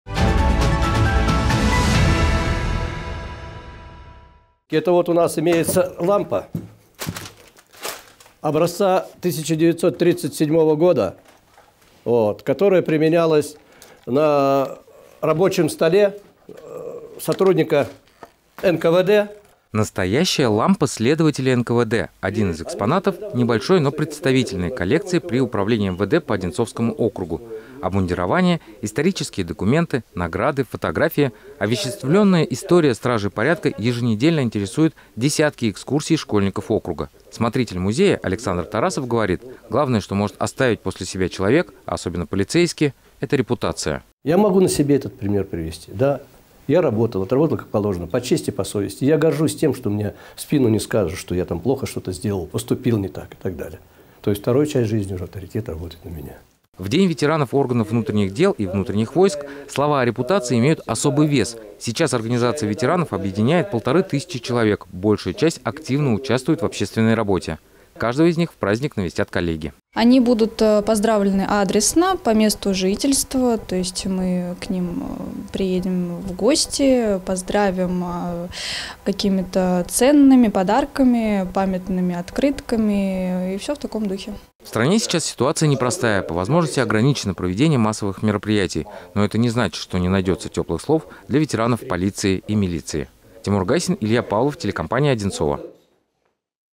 День ветеранов ОВД
Сегодня в России отмечают день ветеранов органов внутренних дел. В отличие от 10 ноября, эта дата не отмечается большими концертами и массовыми мероприятиями, но своих ветеранов полиция не забывает. Репортаж Тимура Гайсина.